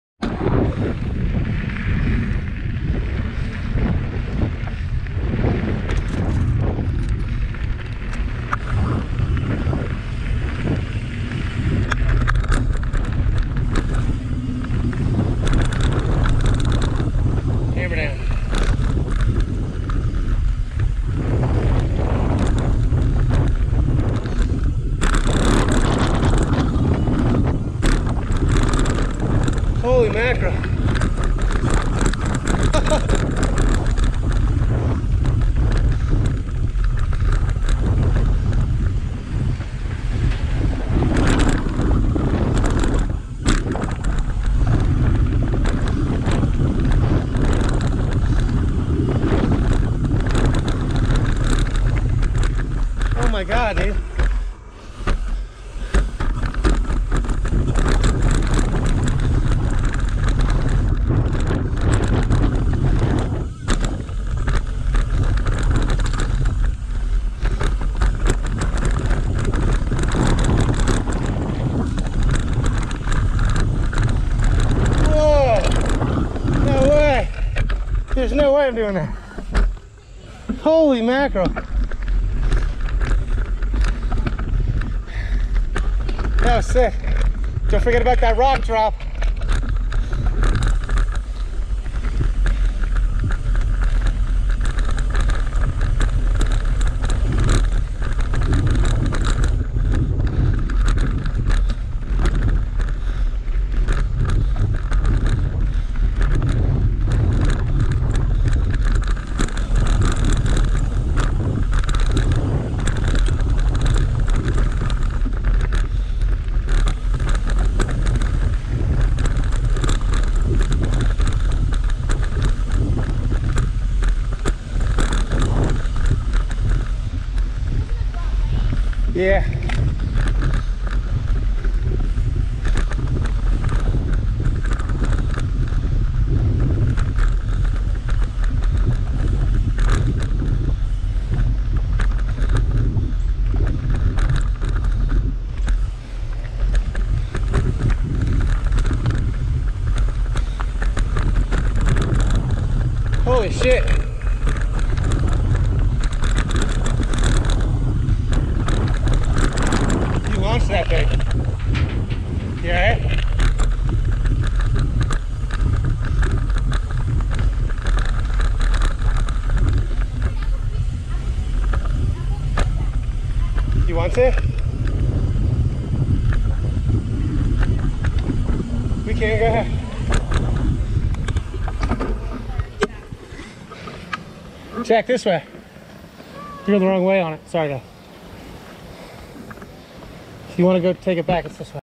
Bentonville Slaughter Pen Town trails- Hammer Down
Crazy fun line with drops n jumps and  rock gardens